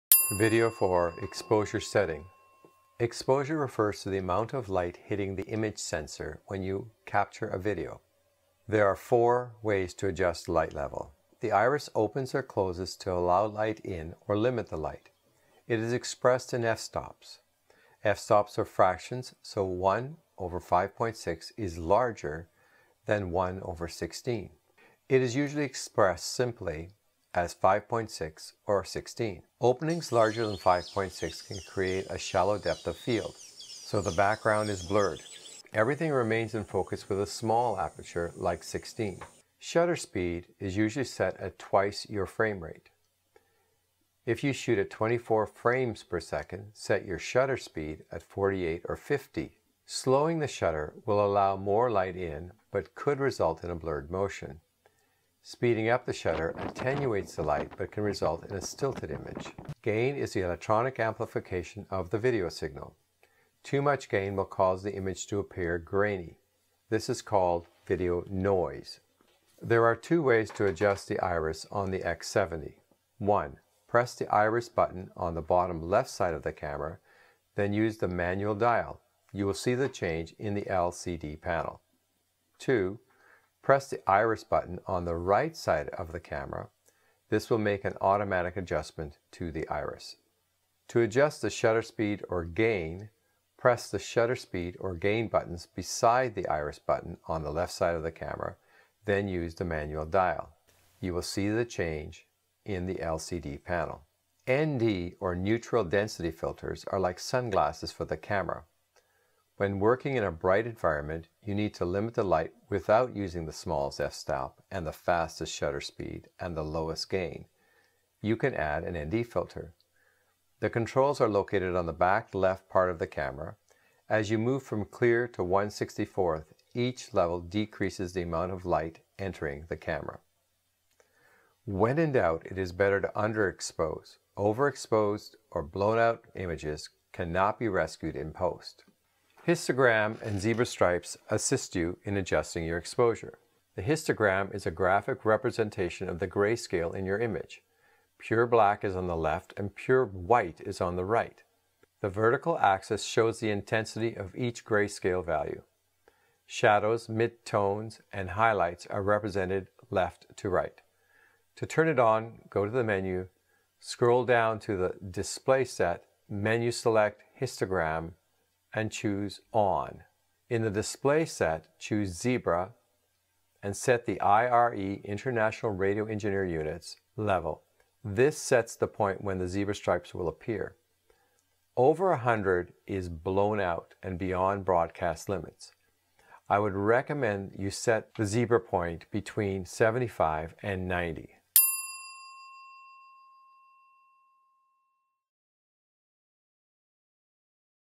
Sony PXW X70 Video 4 Exposure
Learn to manually set the exposure on the PXW X70 camera.